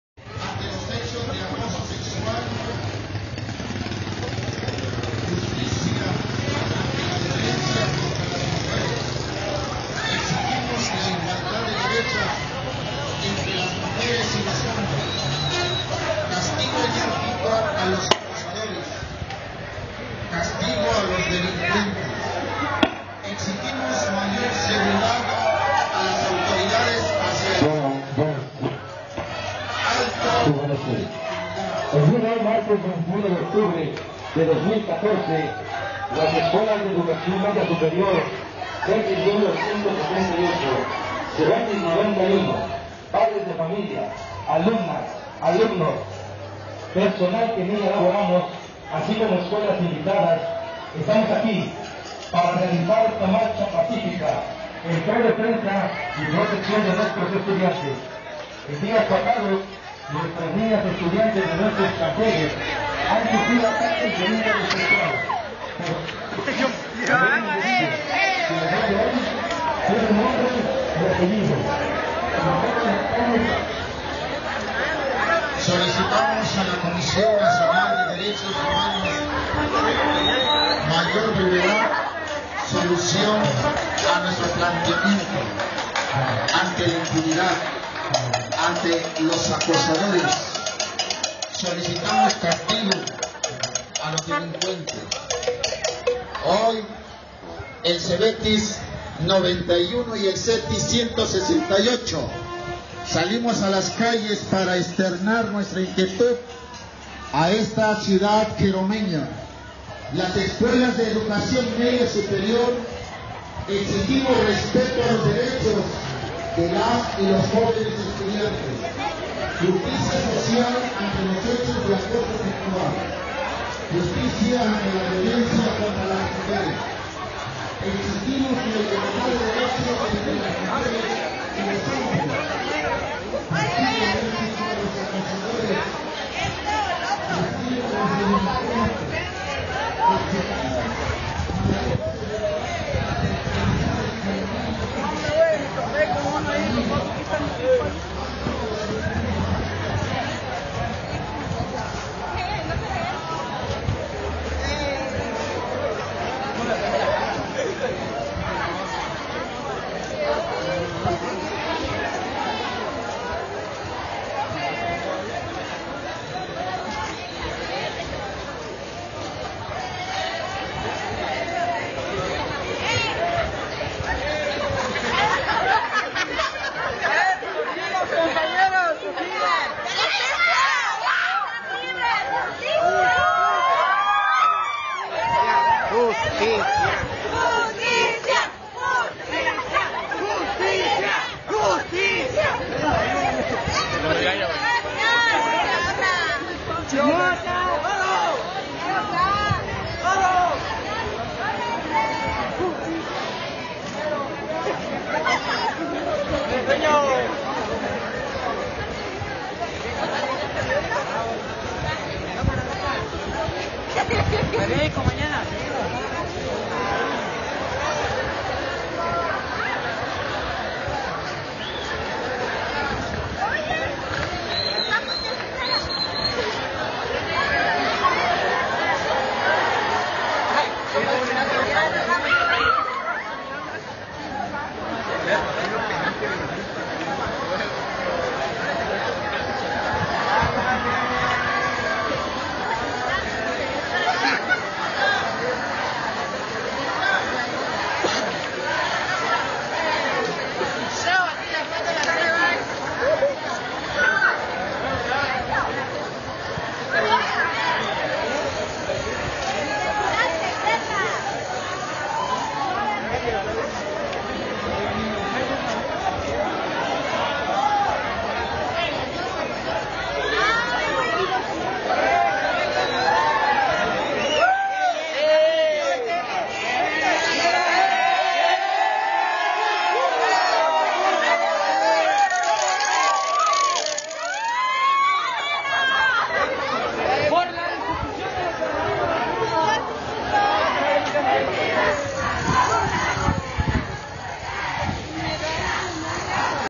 marchan estudiantes preparatorianos pidiendo justicia contra abusador sexual
ante liberación de abusador sexual por parte del ministerio publico, habiendo realizado denuncias respectivas, estudiantes, padres de familia y trabajadores de la educación se manifestaron, tomaron las calles en Ciudad Ixtepec para pedir justicia ya que la policía municipal lo detuvo y el MP lo liberó